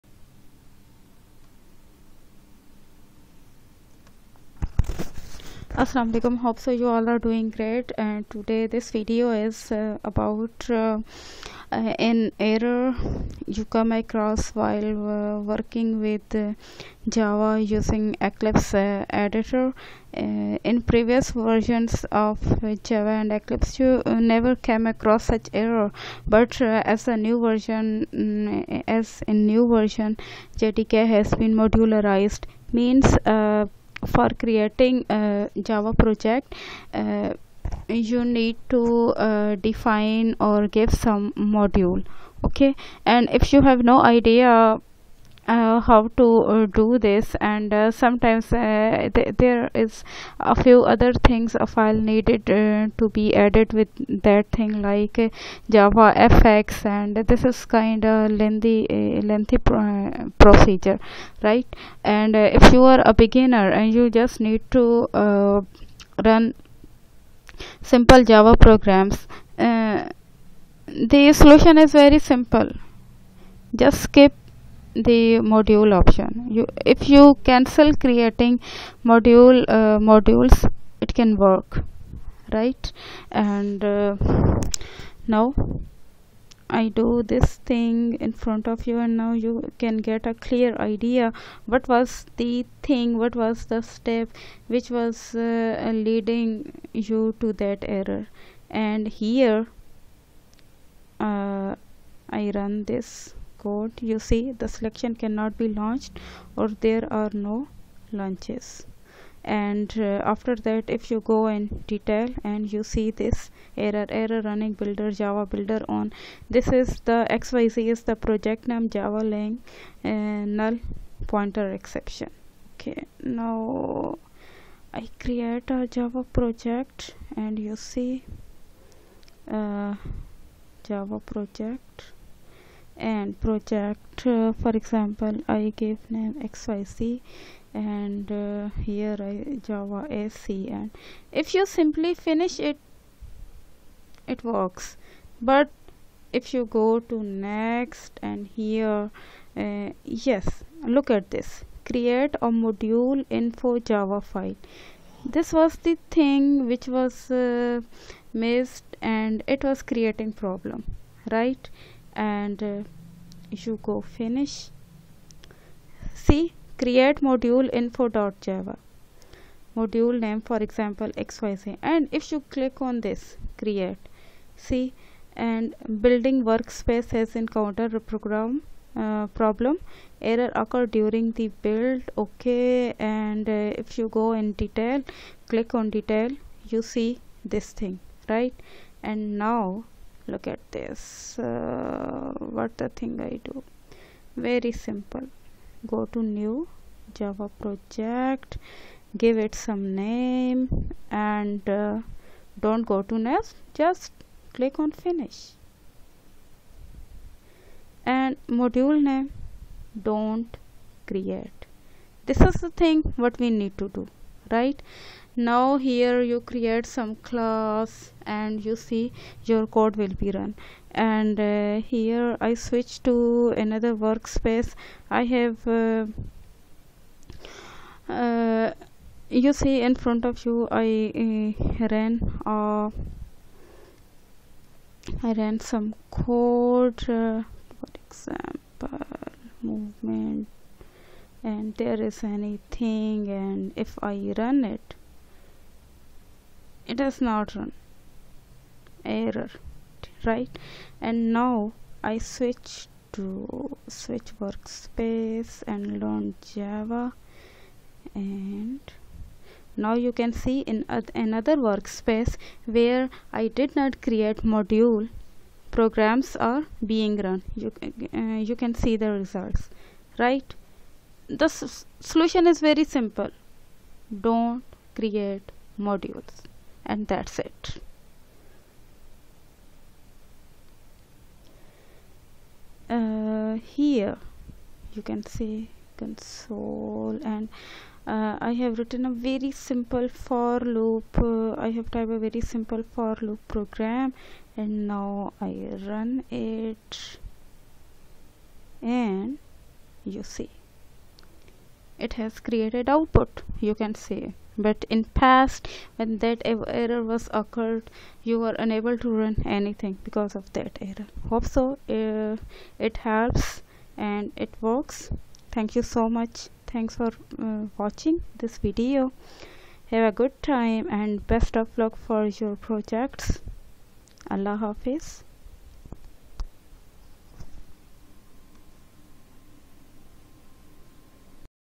Errors running builder 'Java Builder' on project 'xyz'. java.lang.NullPointerException
This video is about an error occurring while trying to run a java program in eclipse. It occurs due to creating a module .